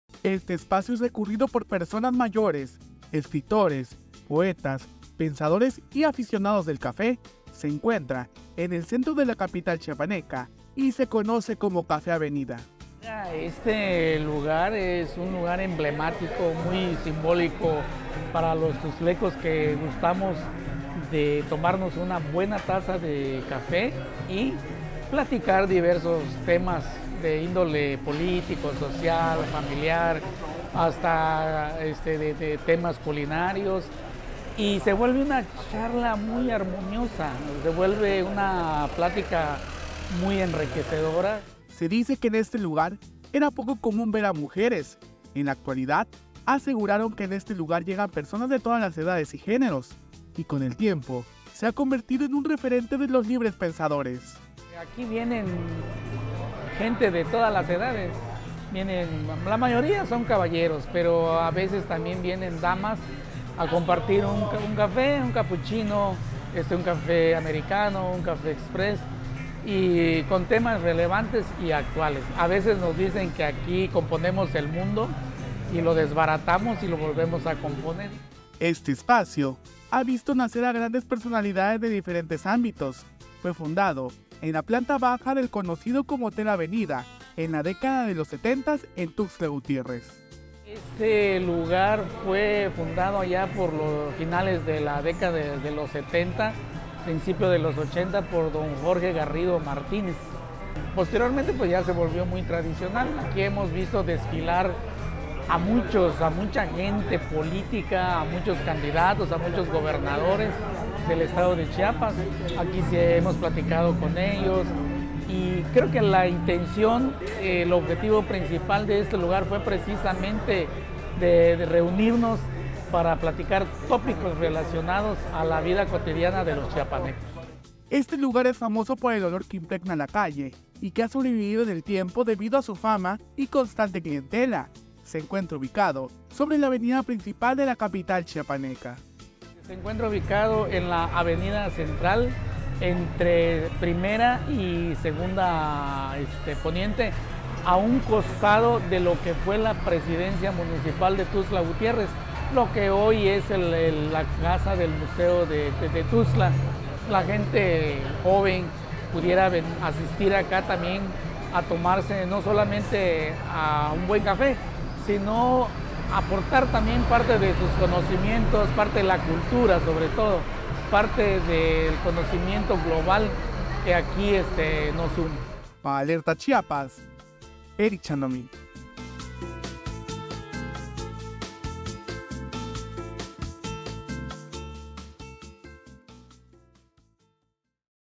Ha visto pasar el tiempo, el Café Avenida de Tuxtla Gutiérrez
La modernidad no ha podido con la historia, por décadas ha sido el punto de encuentro de escritores, poetas, pensadores y aficionados del café.